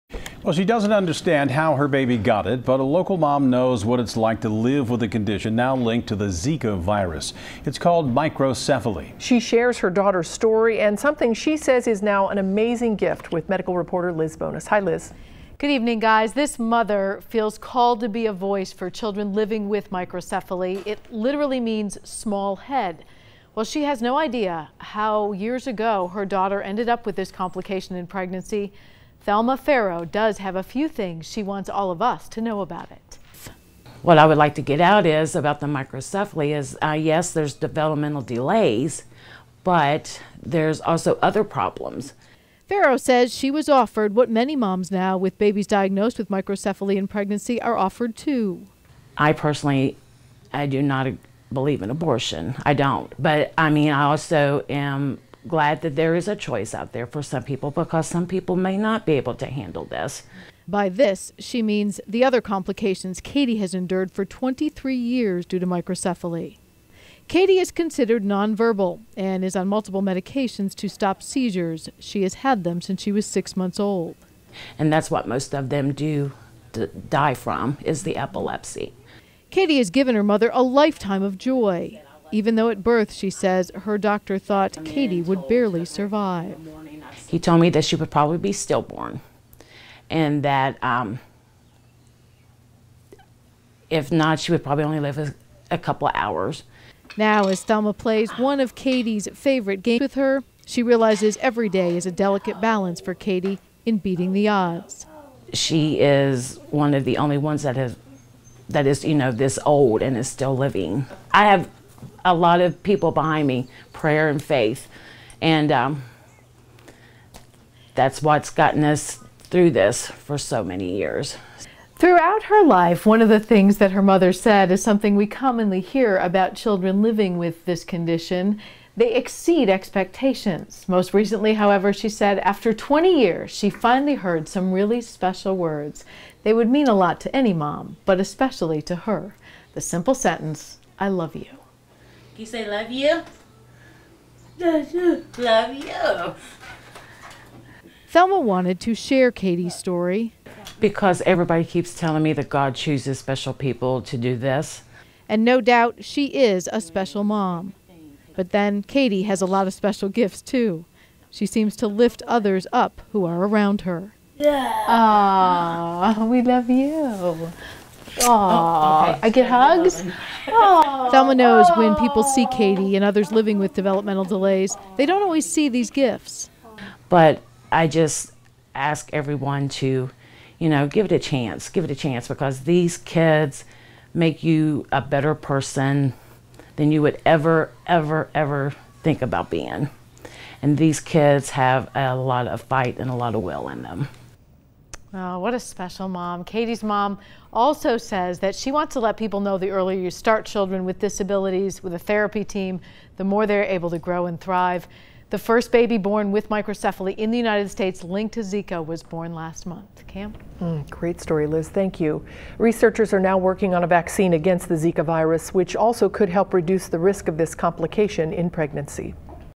Mother shares story of daughter living with microcephaly
CINCINNATI (WKRC) - She doesn't know how her baby got it but a local mom knows what it's like to live with a condition now linked to the Zika virus called microcephaly.

She shared her daughter's story and something she said was now an amazing gift. She felt called to be a voice for children living with microcephaly, which literally means "small head." While Thelma Farrow had no idea how years ago her own daughter ended up with the complication in pregnancy, she did have a few things she wanted everyone to know about it.

"What I would like to get out about the microcephaly is yes, there are developmental delays but there's also other problems," said Farrow.

Farrow said she was offered what many moms now with babies diagnosed with microcephaly in pregnancy were offered too, "I personally do not believe in abortion. I don't but I am glad there is a choice out there, for some people because some people may not be able to handle this."

By "this" she means the other complications Catie has endured for 23 years due to microcephaly. She is considered non-verbal and is on multiple medications to stop seizures she has had since she was 6-months-old.

"That's what most of them do die from is the epilepsy," said Farrow.

Catie has given her mother a lifetime of joy even though at birth Farrow said her doctor thought Catie would barely survive, "He told me that she would probably be stillborn, and if not she would probably only live a couple hours."

Now as she plays one of Catie's favorite games with her, Farrow realizes every day a delicate balance keeps Catie beating the odds.

"She is one of the ones that is this old and still living. I have a lot of people behind me, prayer and faith, that's what's gotten us through this for so many years," said Farrow.

Farrow wanted to share Catie's story, "Because everybody keeps telling me that God chooses special people to do this."

And no doubt she is a special mom and Catie has lots of special gifts that lift others up who are around her.

"But I just ask everyone to give it a chance, give it a chance because these kids make you a better person than you would ever, ever think about being, and these kids have a lot of fight and a lot of will in them," said Farrow.

Catie's mom said she also wanted to let people know the earlier they start any child with disabilities with a therapy team the more they are able to grow and thrive. The first baby born with microcephaly in the United States linked to Zika was born in May 2016. Researchers are now working on a vaccine against the Zika virus, which also could help reduce the risk of this complication in pregnancy.